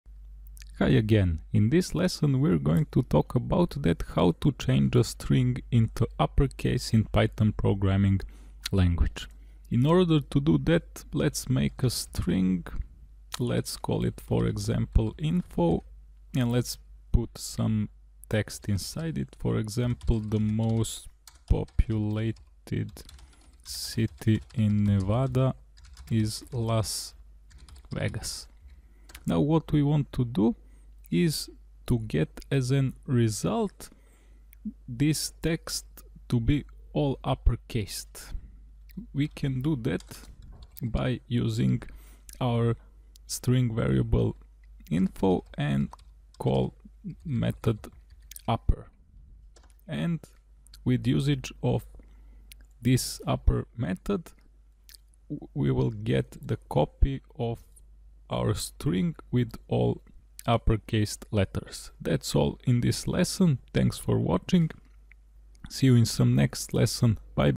How to Change a String into Uppercase in Python programming language uppercase method
In this lesson we're going to talk about that how to change a string into uppercase in python programming language by using upper() method.